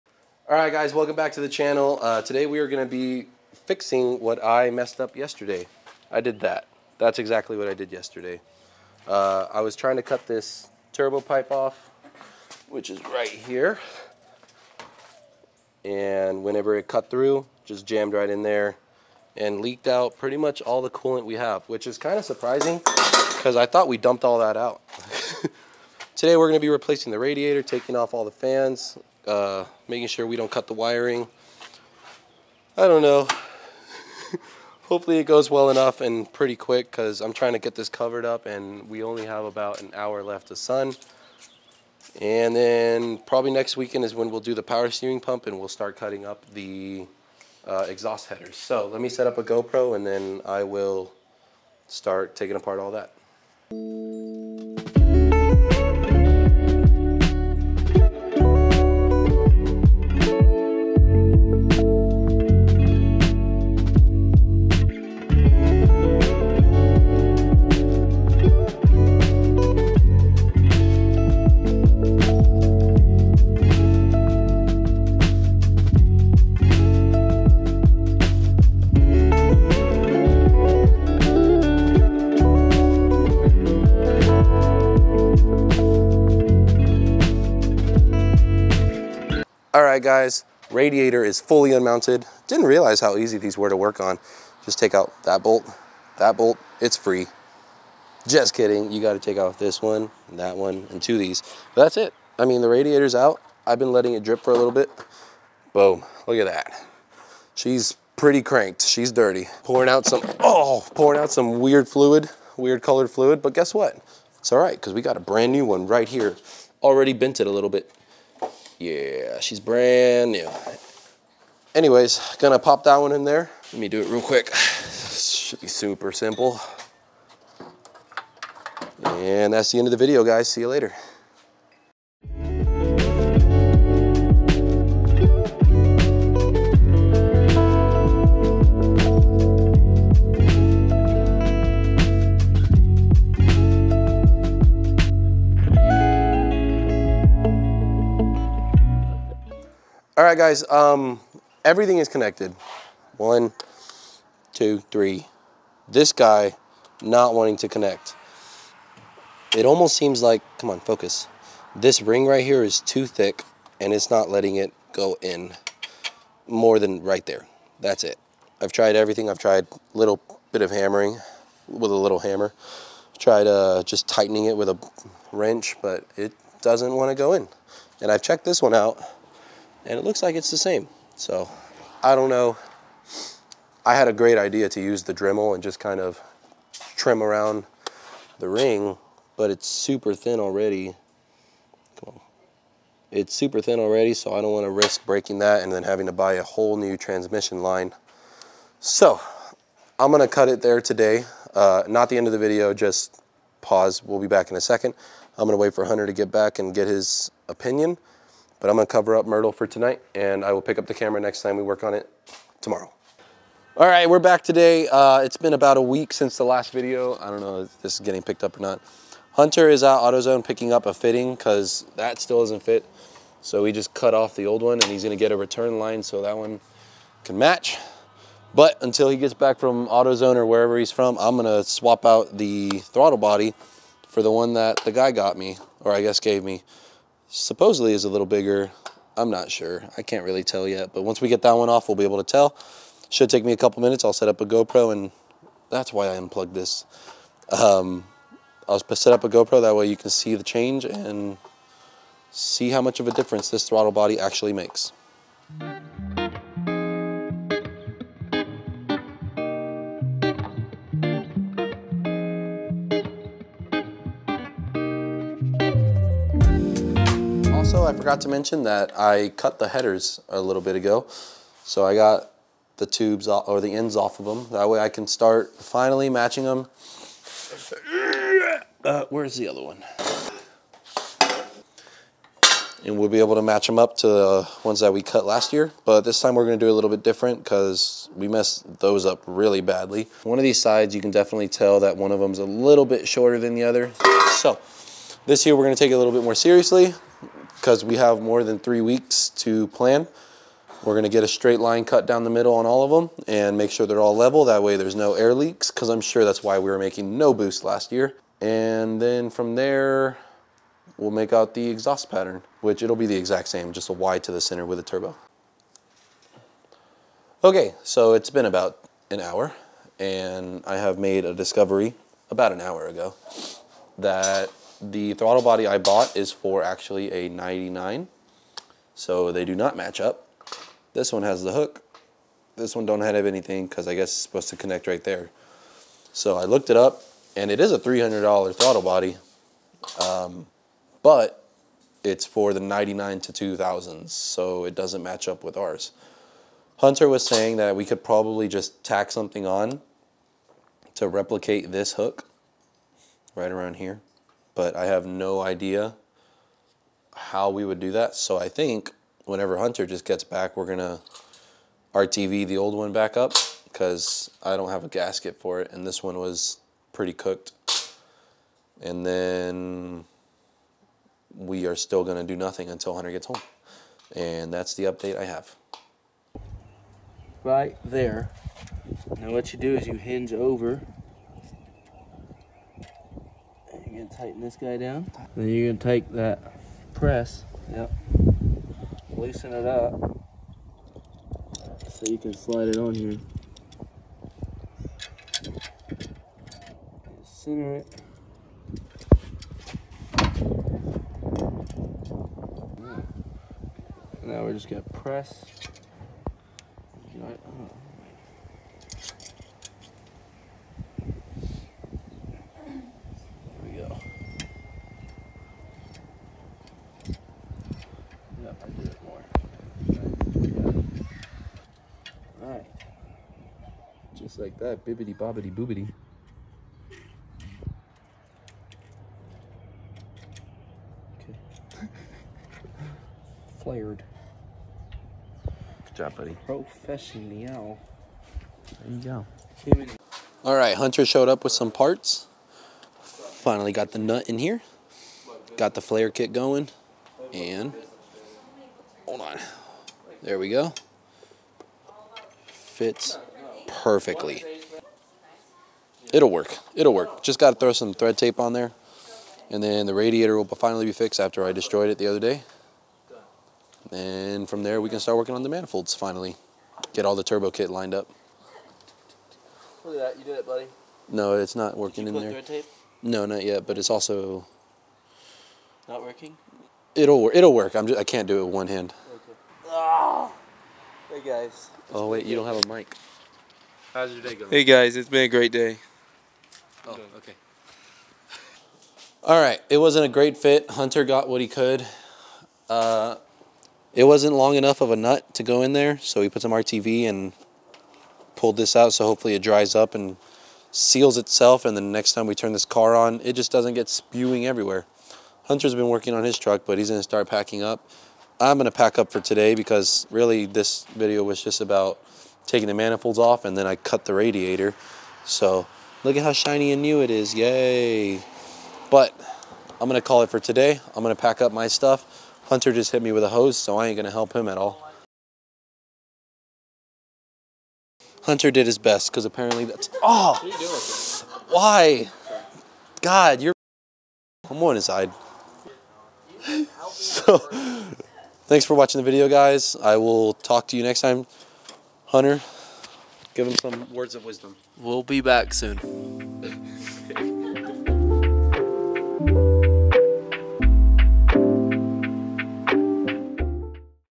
We Got A New Radiator!! Fixing my Mistake!
I bought a radiator and in this short video its replaced. Fixing one of my greatest mistakes in this car, such a rookie mistake.